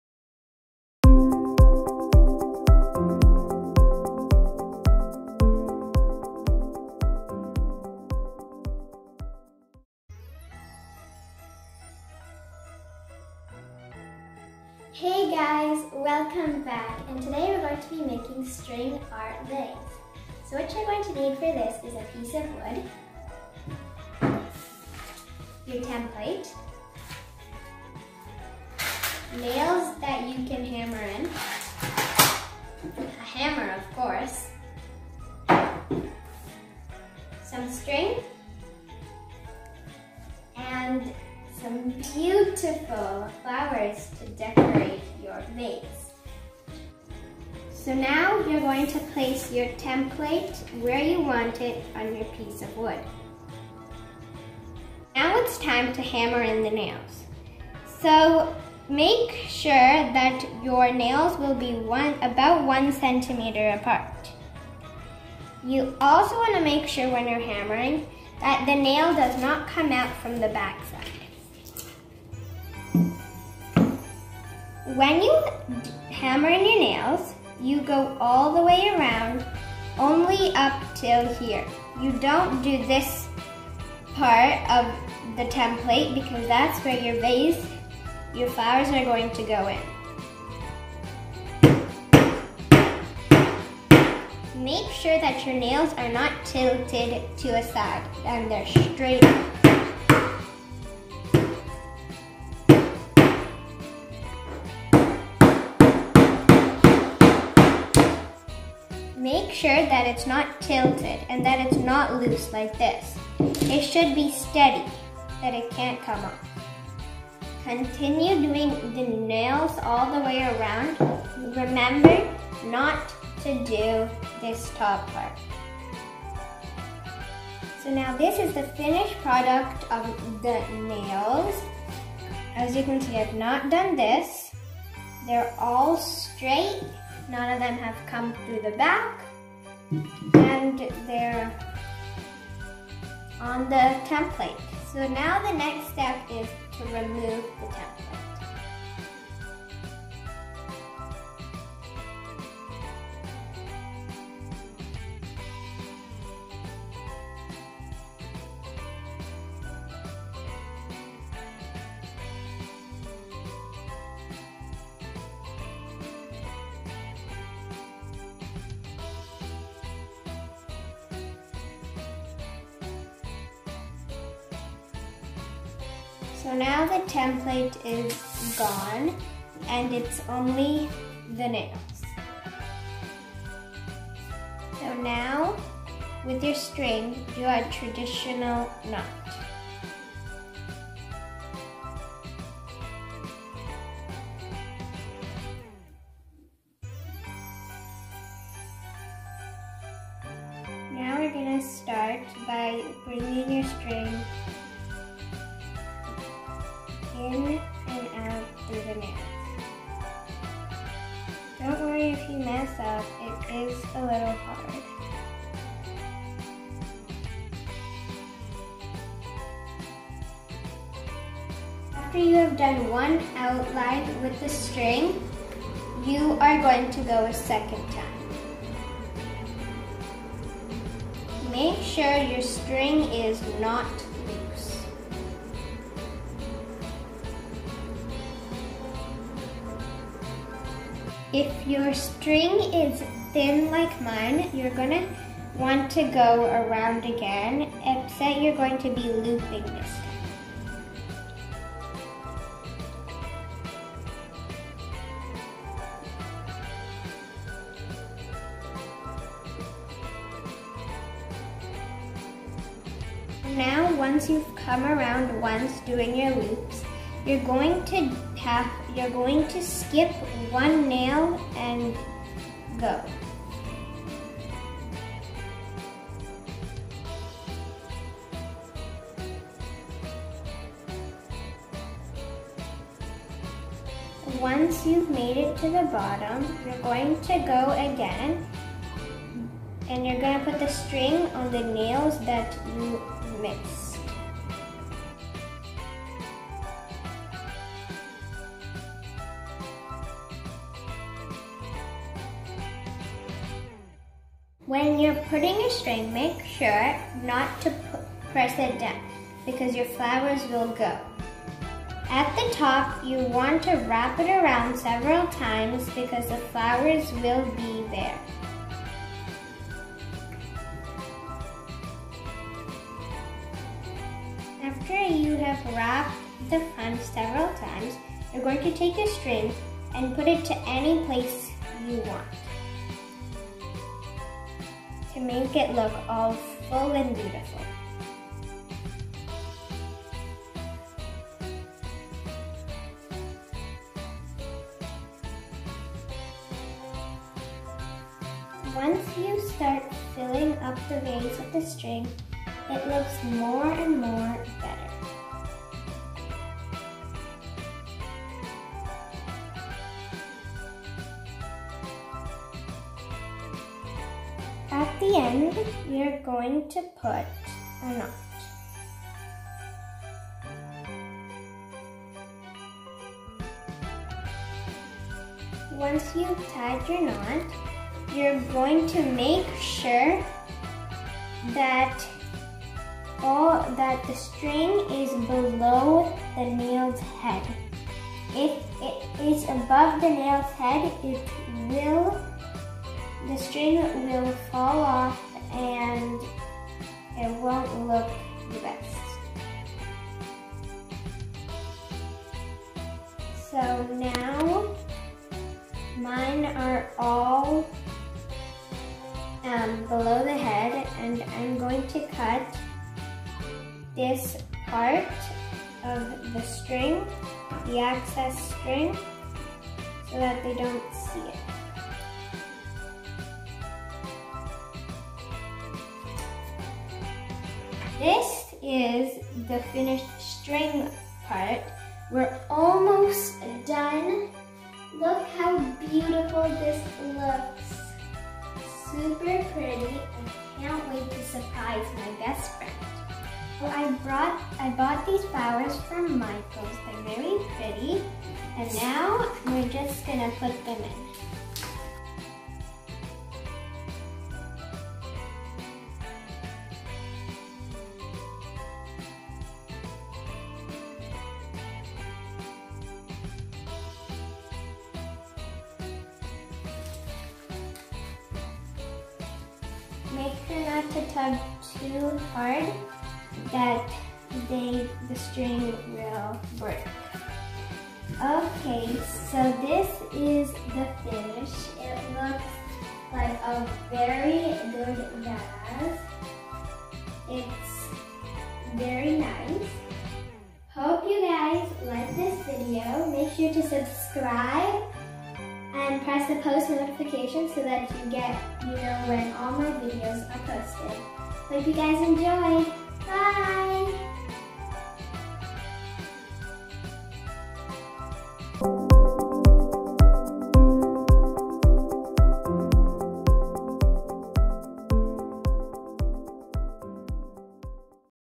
How to make a string art vase
Hey guys so this video is all about how to make a string art vase. Hope you guys enjoy!!